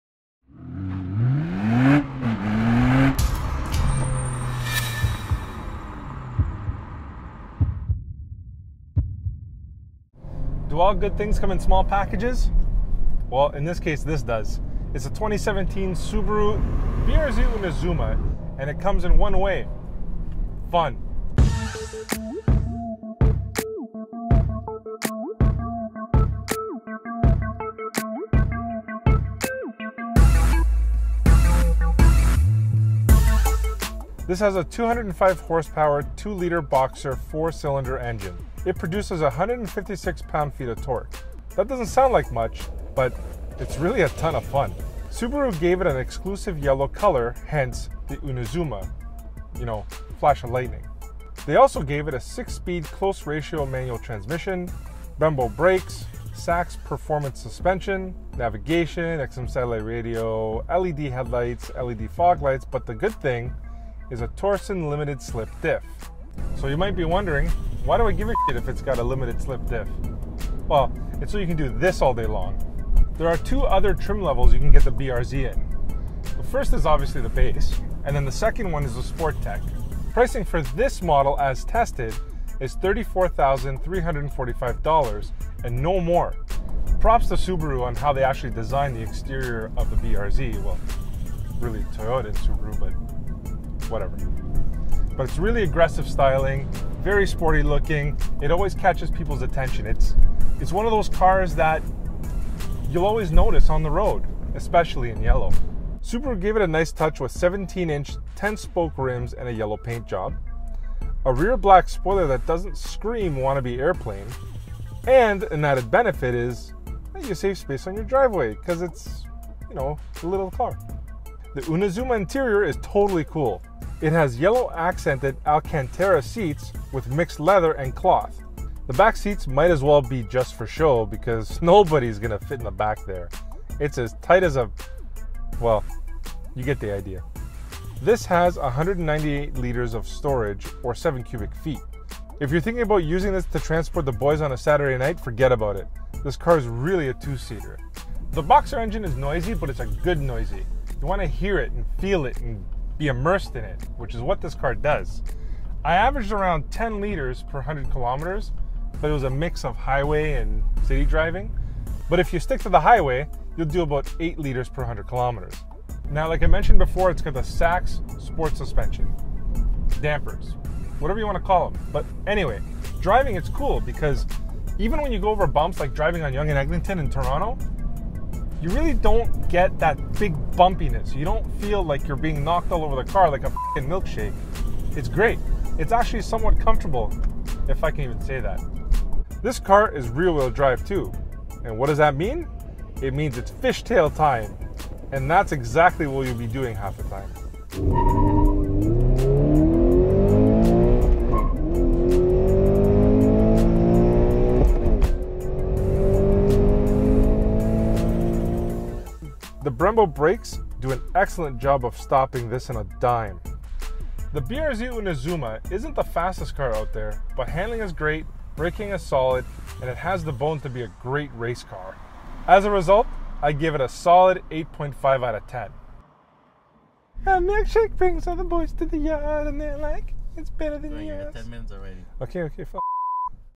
2017 Subaru BRZ Inazuma - Review
Steve sings about a milkshake in this video, also talks about a bright yellow drift capable car.